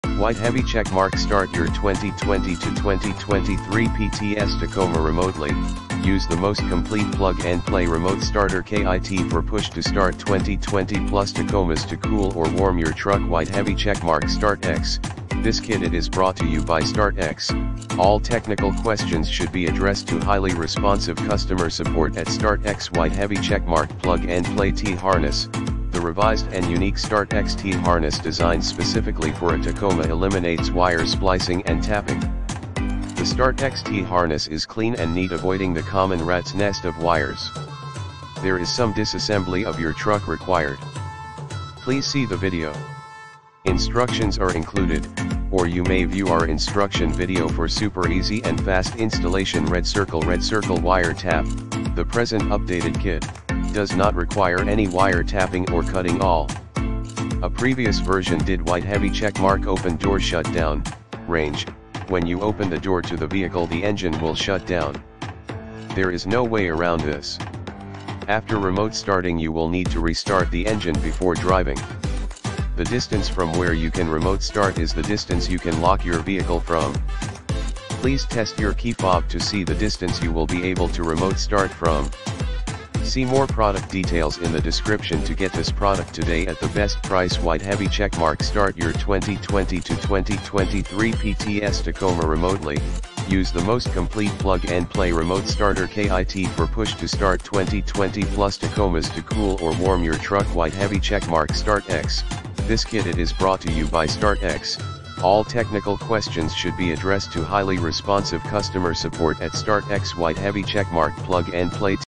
Start-X Remote Starter Kit for 2020-2023 Tacoma Push to Start || 3X Lock to Remote Start || 15 Minu
Start-X Remote Starter Kit for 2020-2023 Tacoma Push to Start || 3X Lock to Remote Start || 15 Minute Install || Zero Wire Splicing!